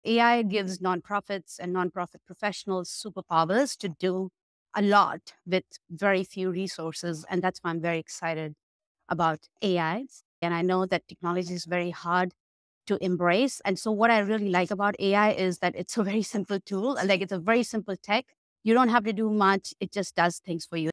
Clip: AI is a simple tool for nonprofits
Harnessing AI for Nonprofit Fundraising: Expert Insights with Deepa Chaudhary

Join Eli and Ben from the TechSoup Connect Western Canada Chapter as they explore AI-driven fundraising strategies with AI expert Deepa Chaudhary, founder of Grant Orb. This session provides insights into using AI tools to create personalized fundraising content, generate impact reports, and optimize nonprofit operations. Learn how to leverage AI's capabilities to maximize your outreach and efficiency, and discover practical AI applications like ChatGPT, Claude, and Grok, among others.

This event was recorded by The Digital Nonprofit on November 5, 2024.